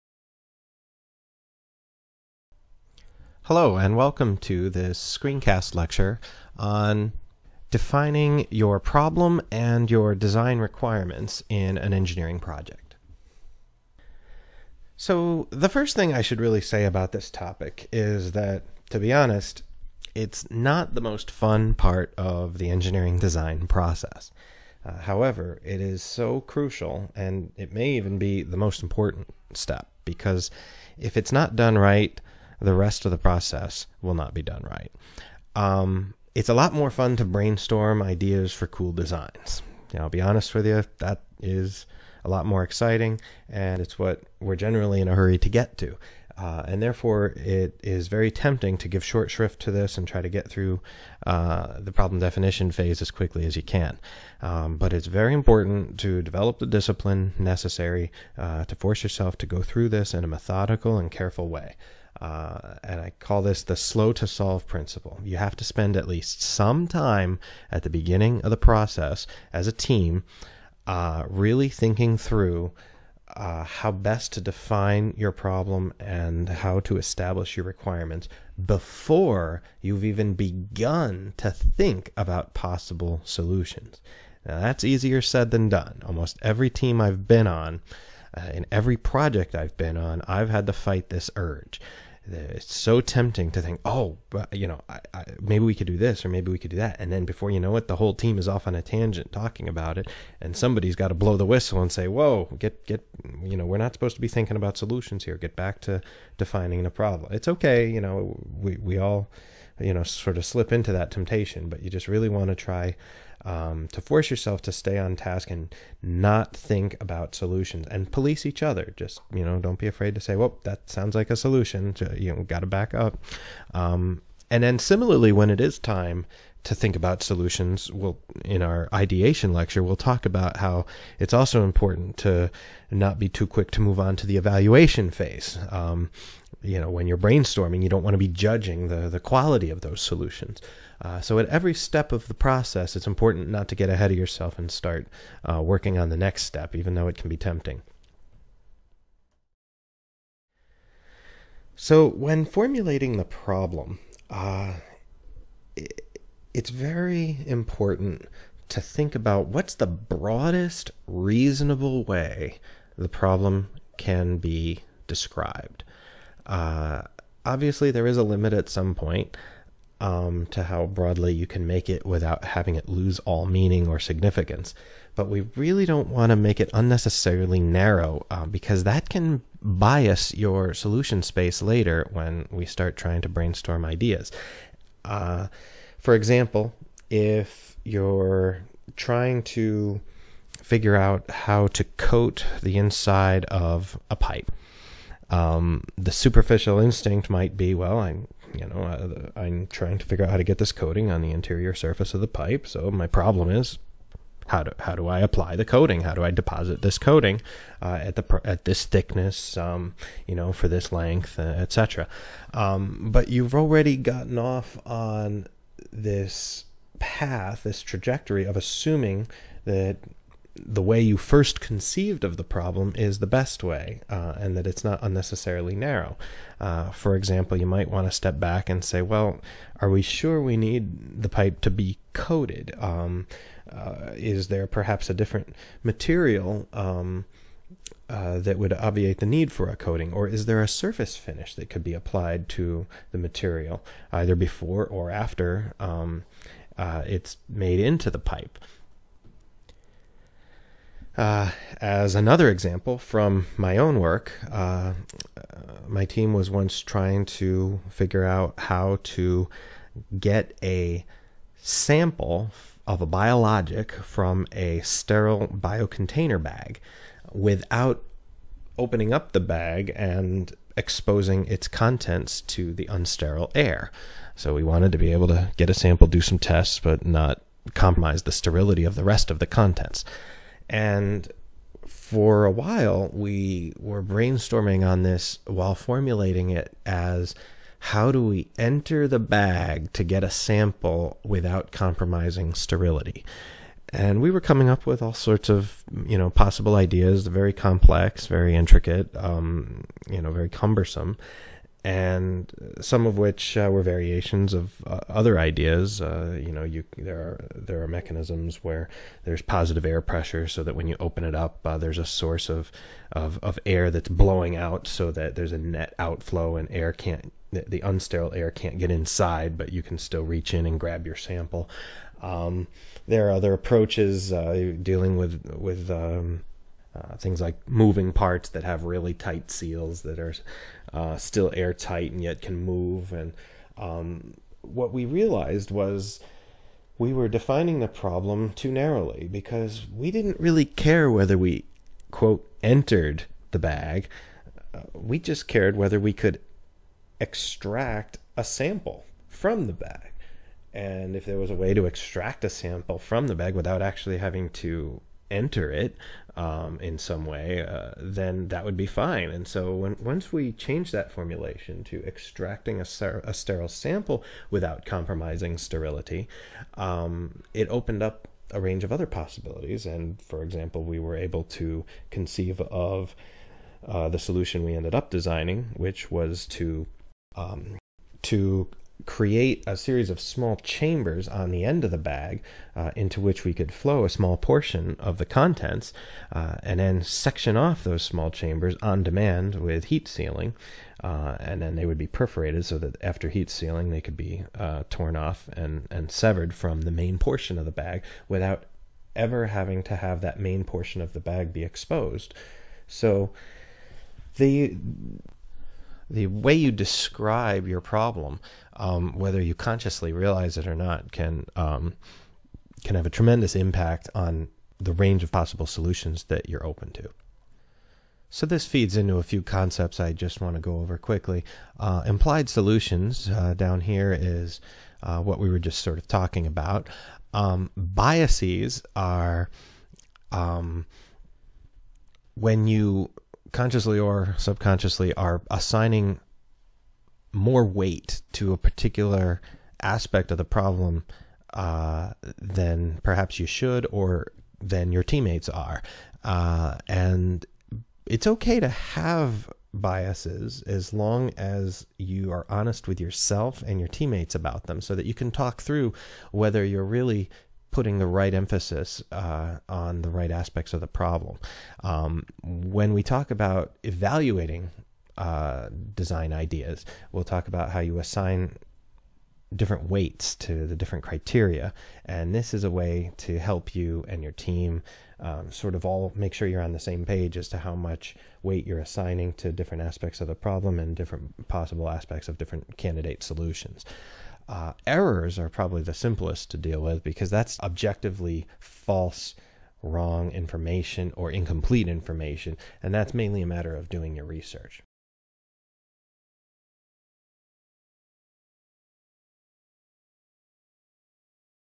Design Requirements Part 1 of 6
1st of 6 screencast segments for "flip" lectures on the topic of problem definition in engineering design.